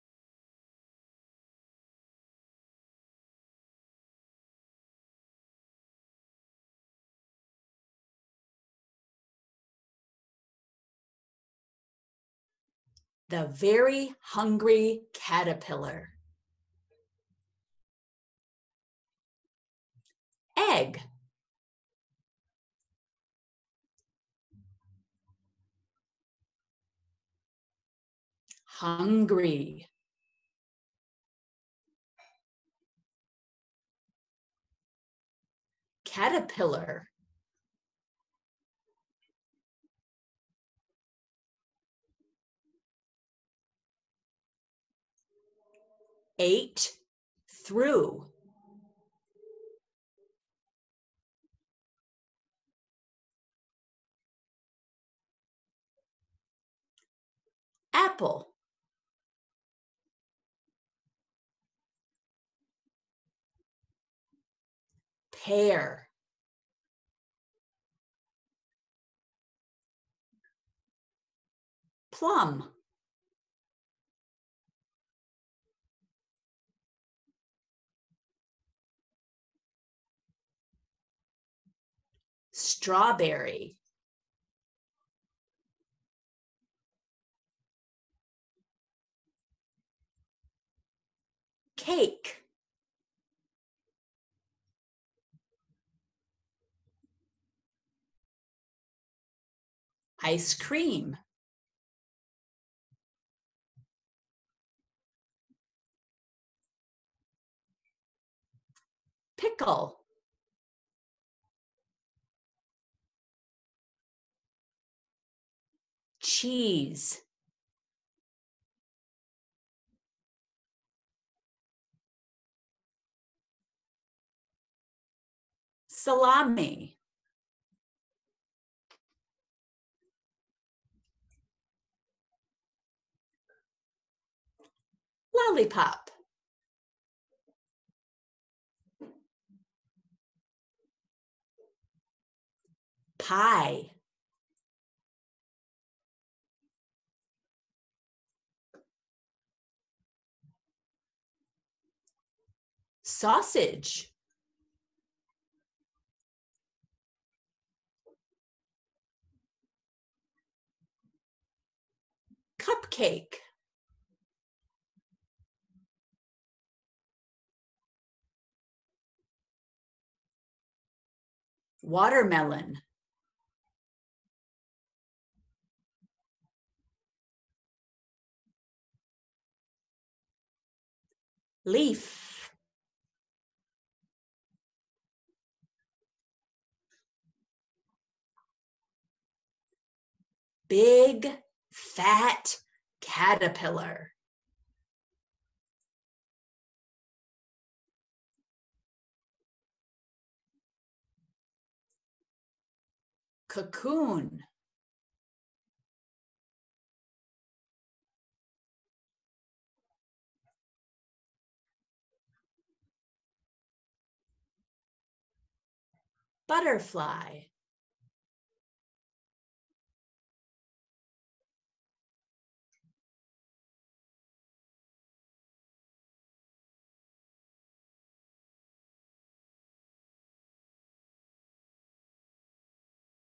The Very Hungry Caterpillar ASL Vocabulary Video
Join DB Central and Central Michigan University's CHARGE Syndrome Research Lab for virtual storybook readings! The Very Hungry Caterpillar is the story of a small caterpillar that eats its way through MANY foods on its way to becoming a beautiful butterfly!

These videos are meant to help you develop your ASL vocabulary and storybook comprehension!